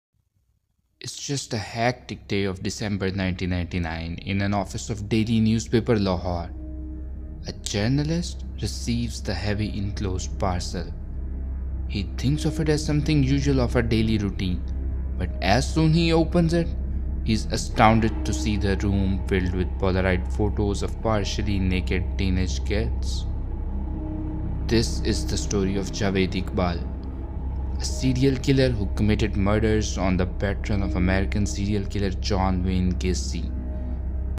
Javed Iqbal Serial Killer - Serial Killer Stories
How many serial killer stories have you heard or serial killer documentaries have you seen, where perpetrator racked up the tally of 100 people?

Javed Iqbal was the Pakistani serial killer, who committed murders of 100 young boys. He confessed to killing and dismembering the bodies. He later dissolved the bodies in an acid and threw them in local Ravi river. His number of kills go way up than Ted Bundy, Jeffery Dahmer, John Wayne Gacy or all of them put together.
Overall, the sheer number of victims and the horrific way in which Iqbal disposed of their bodies, as well as his decision to turn himself in, make his story one of the most unique and chilling examples of serial killing in recent history.